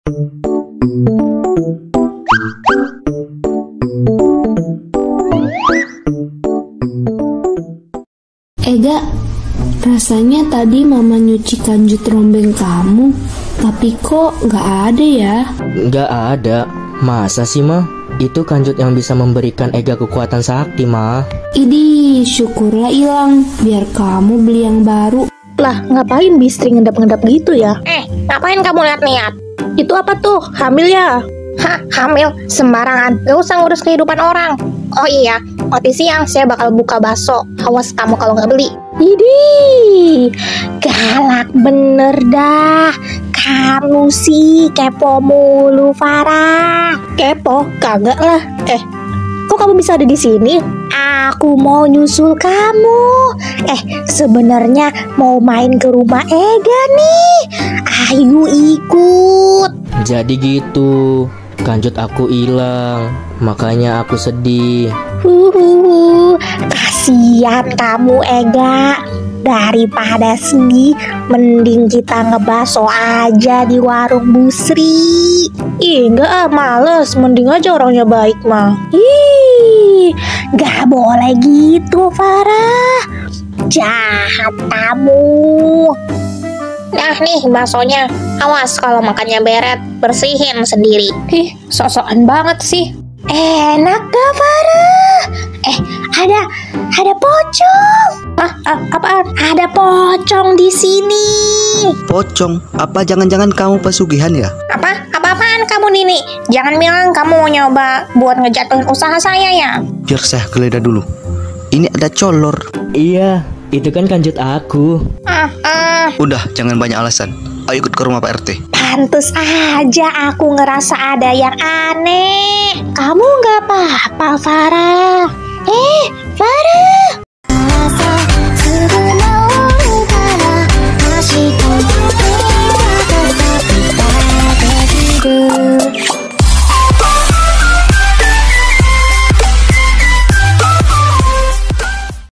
nyolong kanjut untuk pesugihan
guyz ini pelesetan ya...
asal kaya y dari cangcut yg artinya celana dalam, trs diubah ke lanjut:v

animasi lucu ya walaupun ga lucu2 amat wkwkw.

terimakasih kepada teman yg bantu dubing karakter di animasi sbb (credits nama Instagram)
Ega = kikipresetyo
Kiki = Ryan_project1
mamah Ega = Little_starrssssss
Mamat = animasikomik_